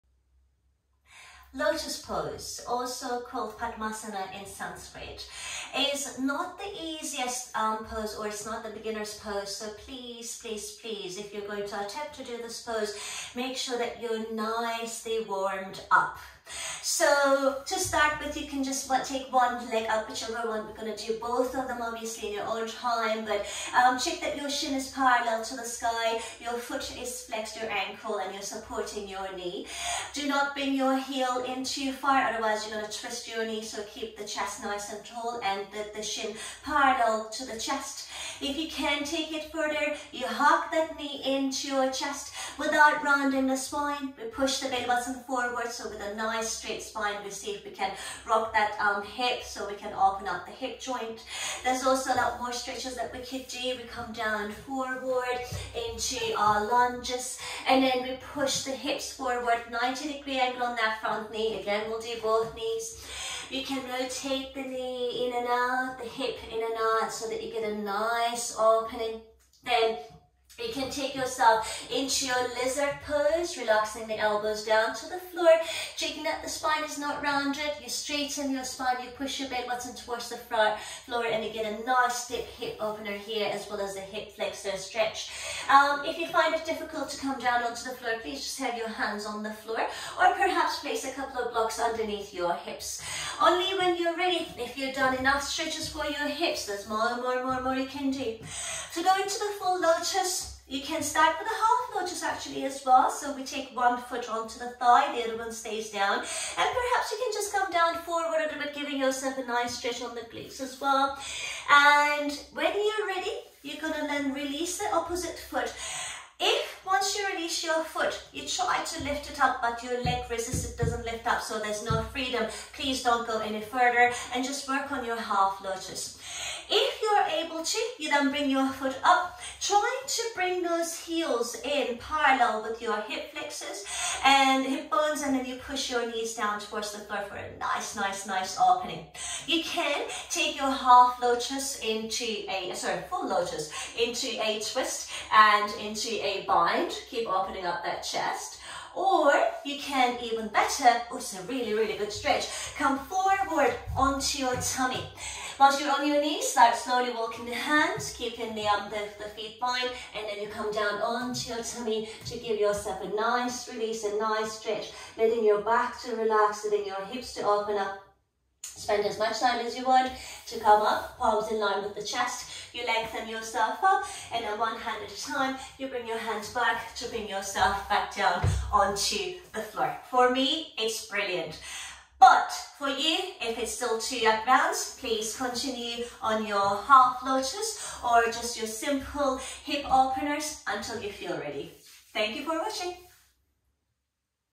Hip Stretches, Yoga Hip Openers, Lotus Pose for Beginners, Advanced Hip Opening Exercises.
There is nothing better than a good hip opener to release both the physical and the mental tension. Sitting all day long, being stressed or generally lacking in movement tighten or hips. Add these stretches in your daily life, soon you will feel the differences. Lotus Pose, or Padmasana in Sanskrit, is an advanced hip opener so if you are tight, try the Half Lotus version first. Last thing you would like to do is injure yourself. Listen to your body's needs first, be patient and enjoy the changes:-)